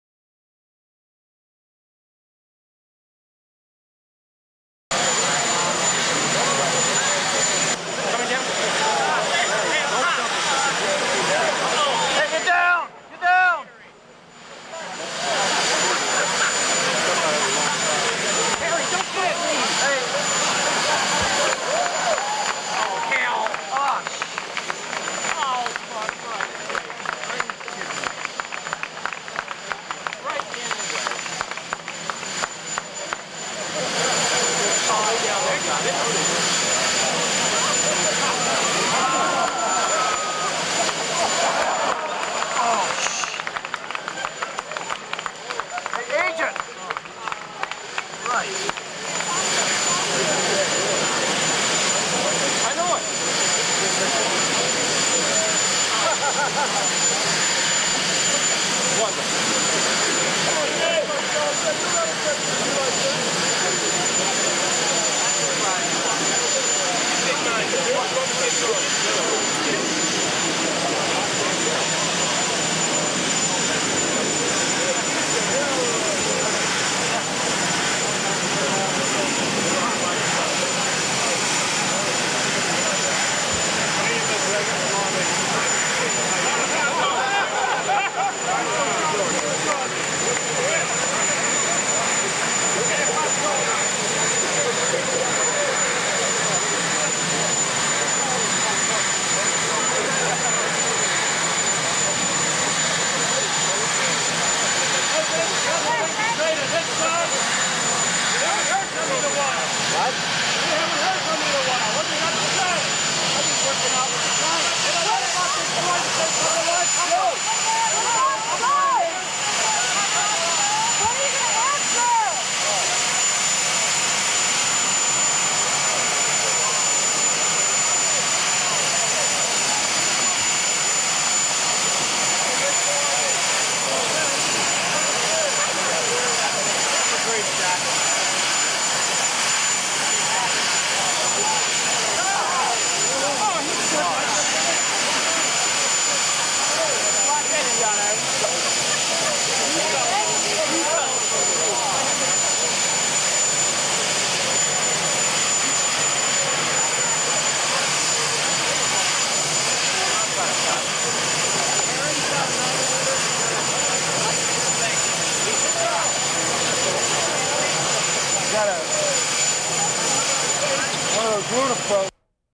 President Reagan's Departure via Marine One Helicopter on February 13, 1987
Full Title: Departure for Camp David. President and Nancy Reagan Depart South Lawn with New York Giants Present. (President Dumping a Box of Popcorn on Harry Carson) on February 13, 1987

Transcript: N/A

Production Date: 2/13/1987

Access Restriction(s):Unrestricted
Use Restriction(s):Unrestricted